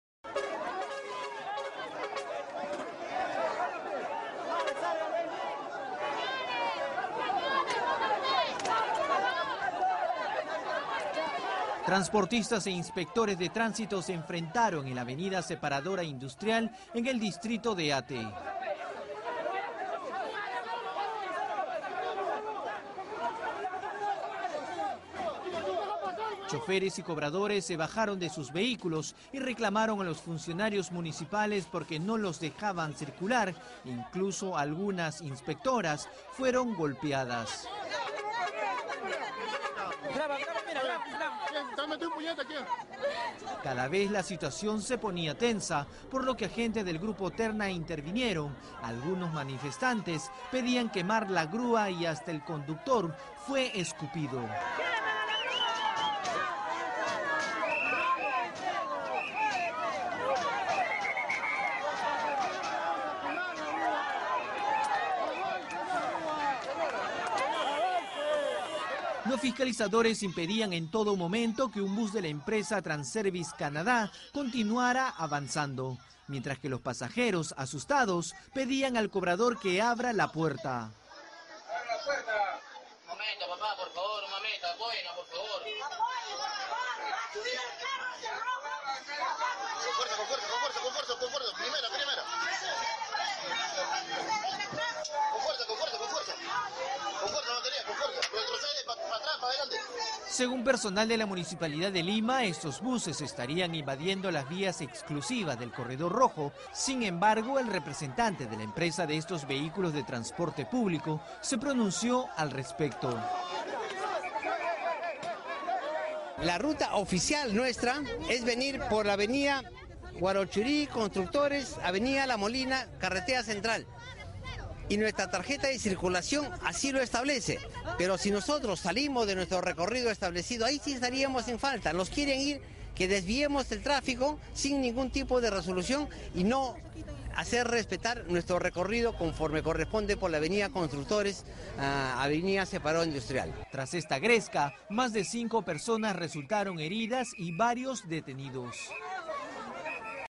Separadora Industrial: operativo contra transportistas informales culmina en gresca
Transportistas e inspectores de tránsito tuvieron una gresca en la avenida Separadora Industrial, en el distrito de Ate. Los choferes y cobradores bajaron de sus unidades para reclamar a los funcionarios municipales porque no los dejaban circular. Algunas inspectoras, incluso, resultaron golpeadas.

Conforme pasaba el tiempo, la situación se tornaba más tensa, por lo que agentes del grupo Terna tuvieron que intervenir. Algunos manifestantes pedían quemar la grúa.

De acuerdo al personal de la entidad edil, los buses de transporte público estarían invadiendo las vías exclusivas del corredor rojo. Sin embargo, el representante de la empresa de transportes Transervice Canadá, Ricardo Pareja, negó que estén infringiendo normas de tránsito y aseguró que si salen de su recorrido establecido, ahí sí estarían incurriendo en falta.

Tras el conflicto, más de cinco personas resultaron heridas y hubo varios detenidos.